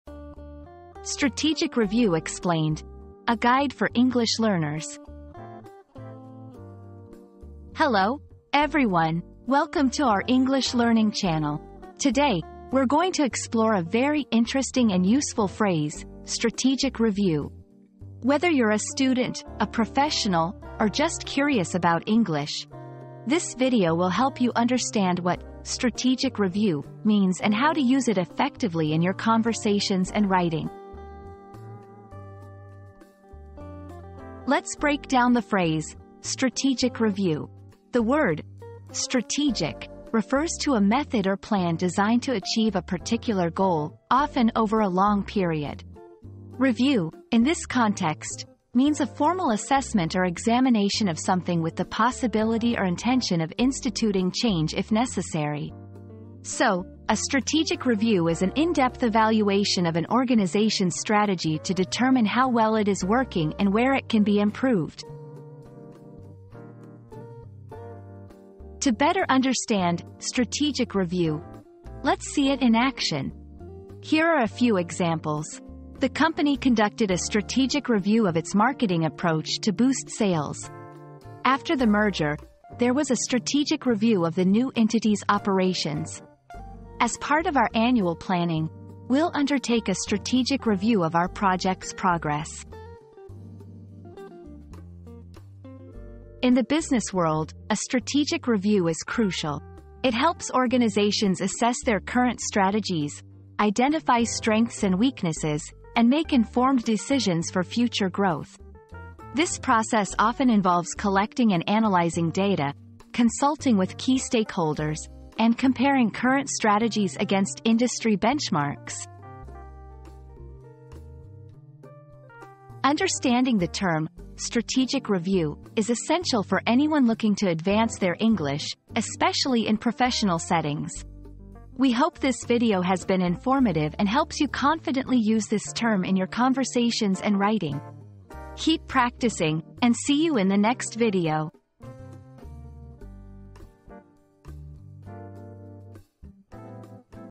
Strategic Review Explained: A Guide for English Learners
Mastering the Strategic Review: A Guide for English Learners • Unlock the secrets of the Strategic Review process with this comprehensive guide designed specifically for English learners. Discover proven strategies and techniques to excel in strategic reviews and enhance your professional skills. Don't miss out on this invaluable resource!

00:00 • Introduction - Strategic Review Explained: A Guide for English Learners
00:33 • Understanding the Phrase "Strategic Review"
01:08 • Examples of "Strategic Review" in Sentences
01:35 • The Importance of "Strategic Review" in Business and Management